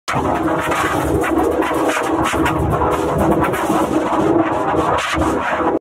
(CHANGED^2) Klasky Csupo in Videoup V82
Changed, because Jayden Klapof forgot instructions.
Video: Videoup V8.3.VOCODEX + G-Major 13 + Videoup V7.NONE + Audioup V10.NONE
Audio: AM Radio + Sad Waves + Ringtoneup V3.VOCODEX